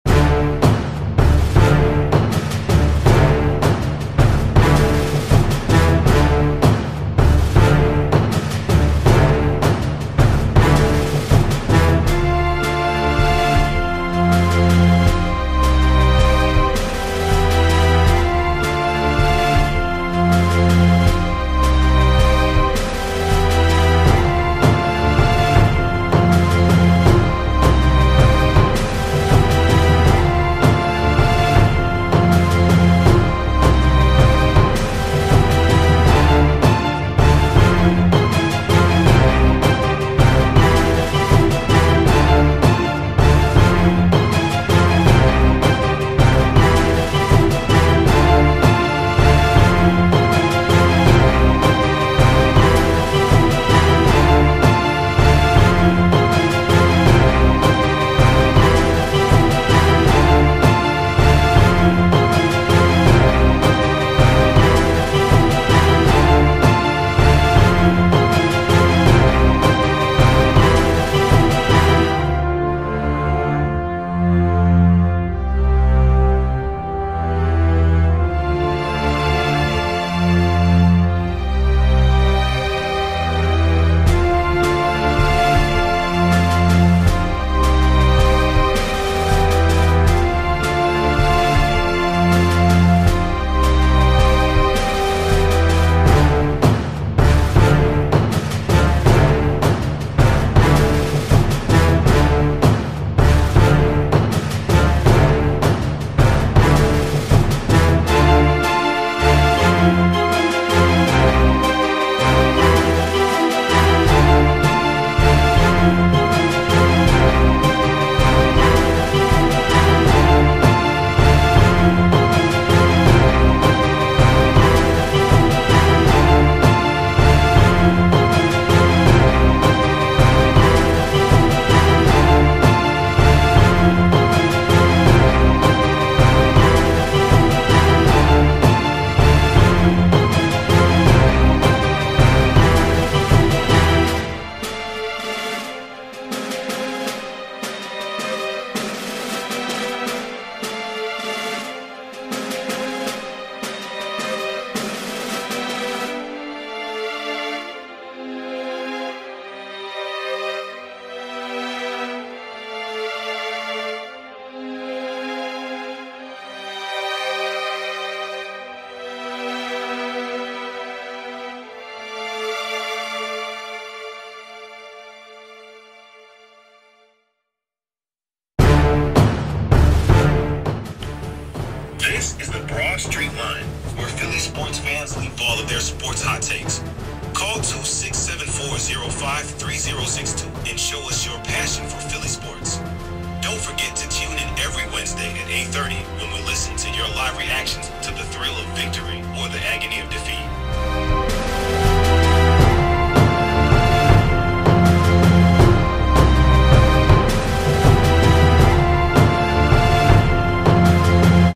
vs @MemphisGrizzlies @NBA Highlights 02.23.23
@sixers beat the @MemphisGrizzlies  110-105
@phiredupsports Podcast Network
Video Credit- @NBA